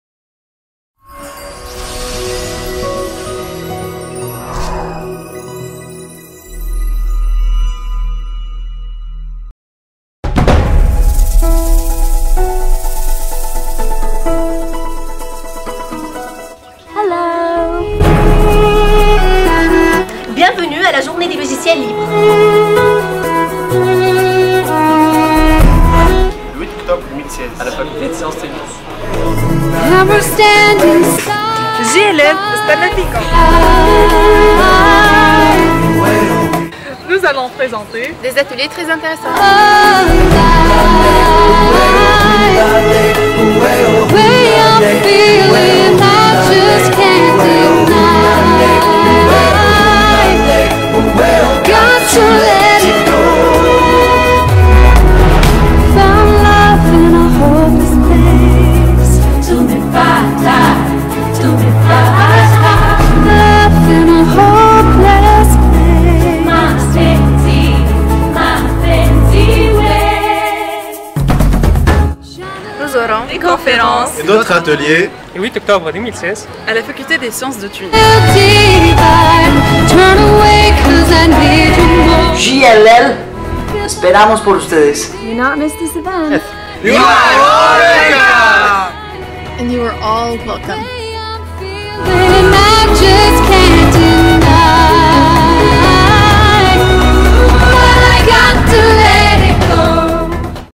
teaser JLL
Pour célébrer encore une année de succès, notre grande famille du Club des Logiciels Libres sera honorée par votre présence
Le 8 octobre 2016 à la faculté des sciences de Tunis pour assister à la journée des logiciels libres ou vous attendent DES SURPRISES et des ACTIVITÉS diverses non seulement pour apprendre,cotoyer des experts et développer vos connaissances mais encore pour communiquer,s'amuser et BRISER LA ROUTINE !!
Joignez donc l'utile a l’agréable et soyez nombreux

Réalisateur: Turki Mahmoud
Cameraman: Belaiba Oussama
Montage: Baklouti Ameni, Nouili Khalil
Acteurs: Michelle, Baklouti Ameni, Rmadi Khouloud, Lamine Zainab, Mokdad Chahra, Attia Amine, Chabchoub Karim, Ben Slama Hassen,Krifa Ibtihel, Maatoug Dhouha, Rakrouki Hamza, Slama Iyed, Mahjoub Hajer, , Halleb Samira, Belabed Jihen, Gasmi Zied, Ayech Mohamed, Suarez Barco Ricardo, Anna Julie